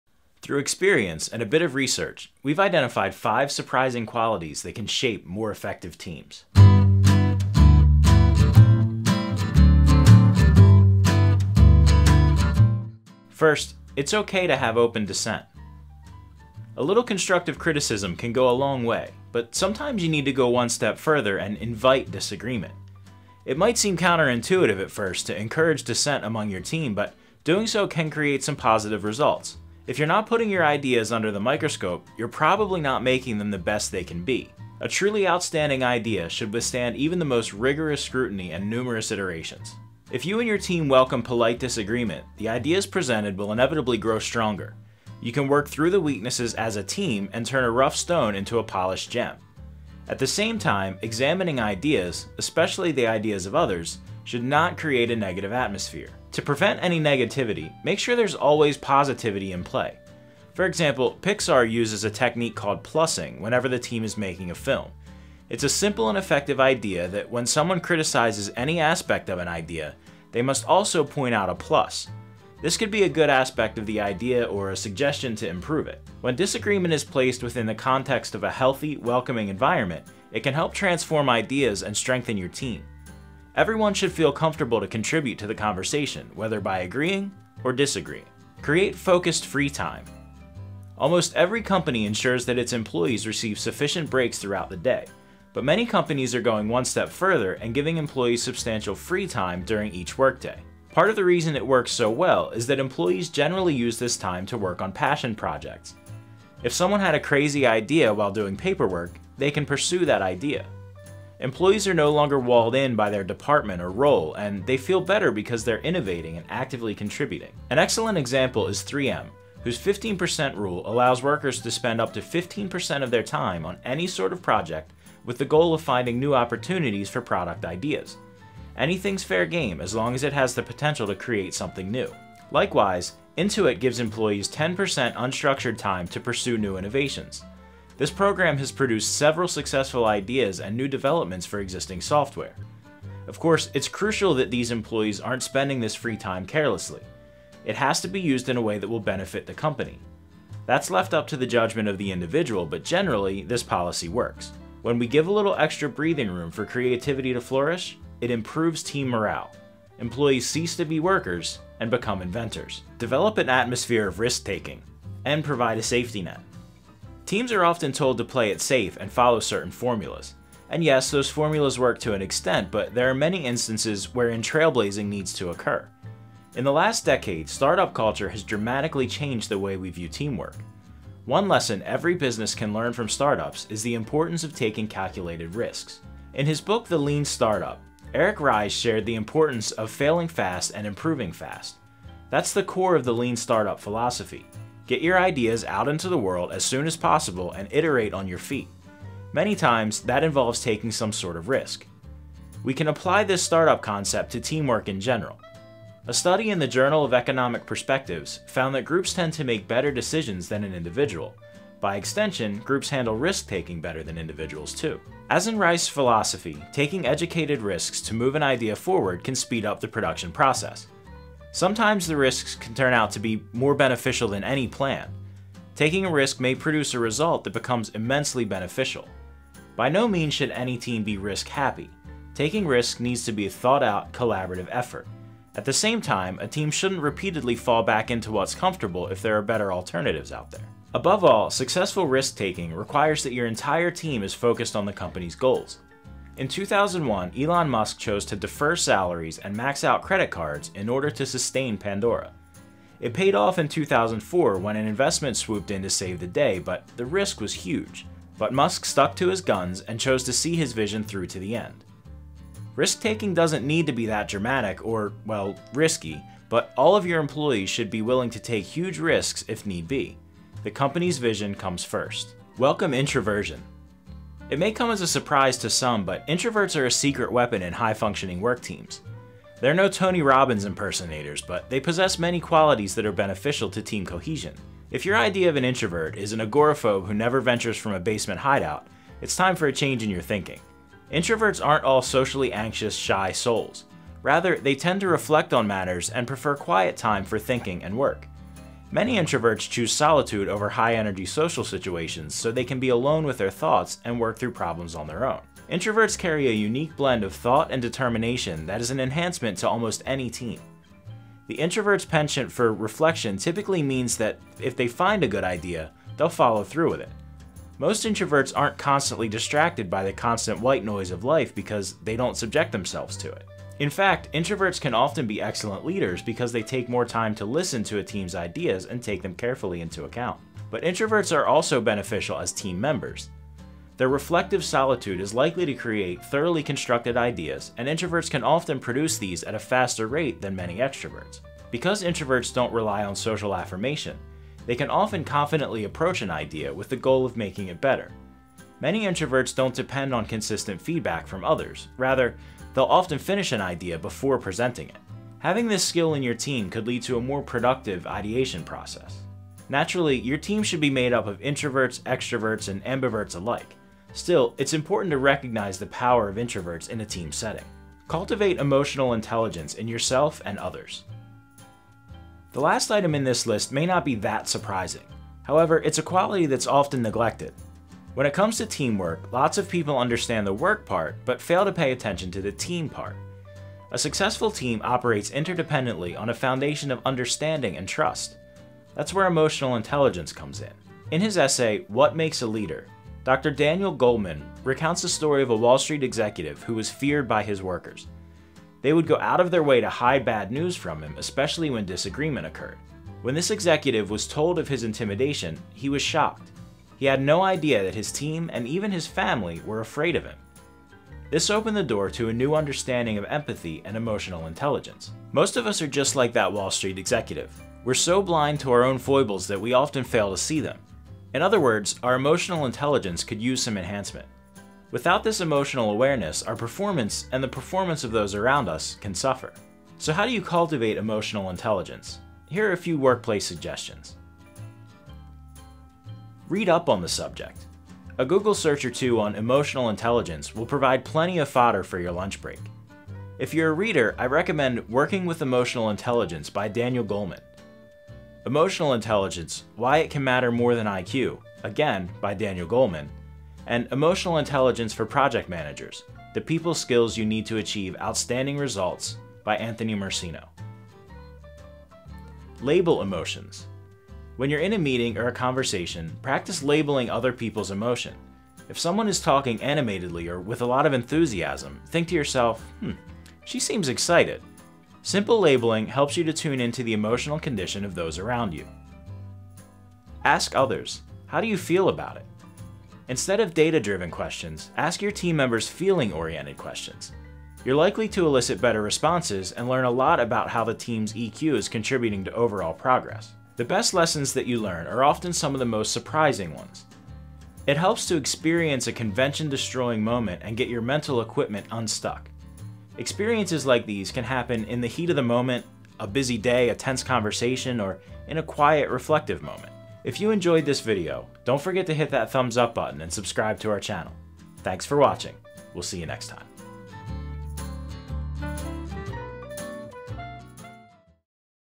5 Surprising Qualities That Can Shape Very Effective Teams
Through experience and a bit of research, we’ve identified five surprising qualities that can shape more effective teams.